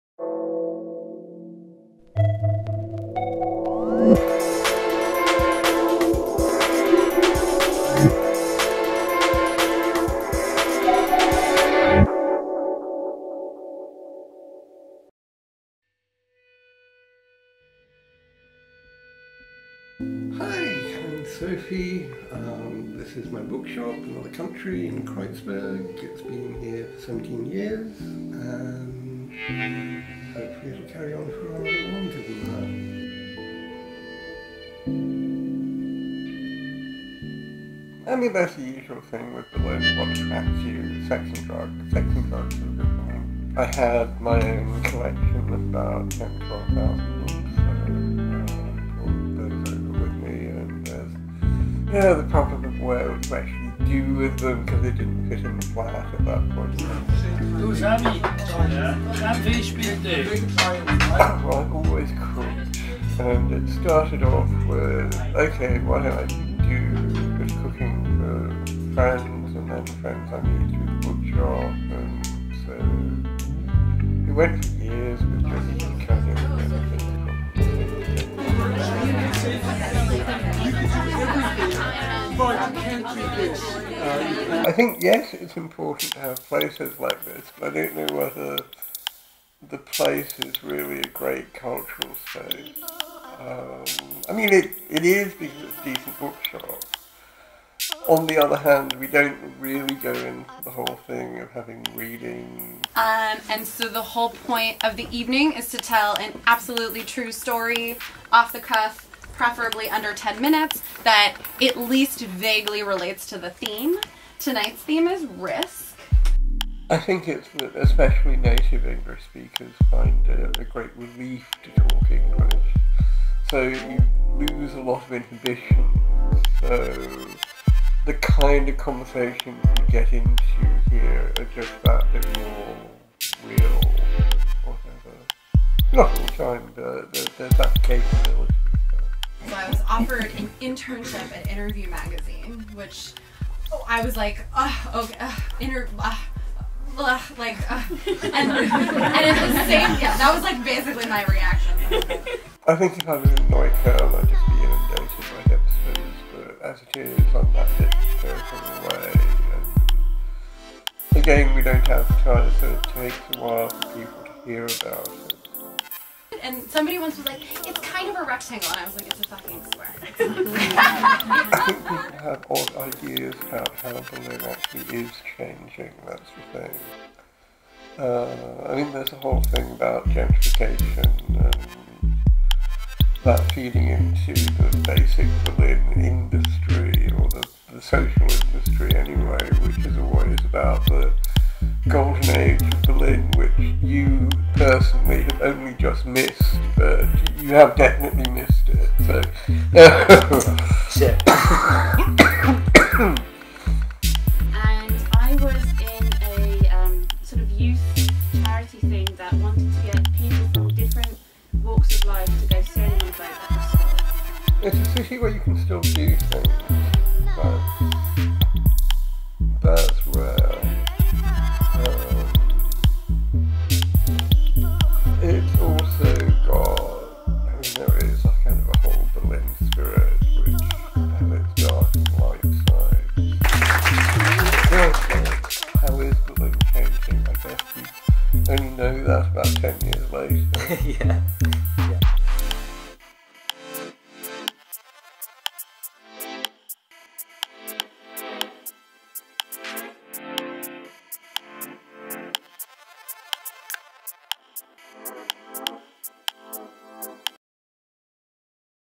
Odd City Episode Three: Cooking the Books in Kreuzberg
In the third episode of Odd City Berlin, we visit a little-known sanctuary for English-speaking expats living in Berlin; a home away from home where visitors can curl up with a glass of wine and a good book, tell humiliating stories to a room full of strangers and gorge on homemade comfort food.

We meet owner Sophie Raphaeline, a native Brit who serves traditional English roast dinners at the Kreuzberg bookshop she opened 17 years ago. Another Country boasts tens of thousands of English-language books, monthly story telling sessions and a unique atmosphere for a second-hand bookshop, and with Sophie we explore gentrification, expat living and a need for cultural spaces in large cities.

Odd City is a collaborative series by filmmaker James McBreen and his production company Overland Originals; Grover, a platform supporting creatives; and LOLA Magazine.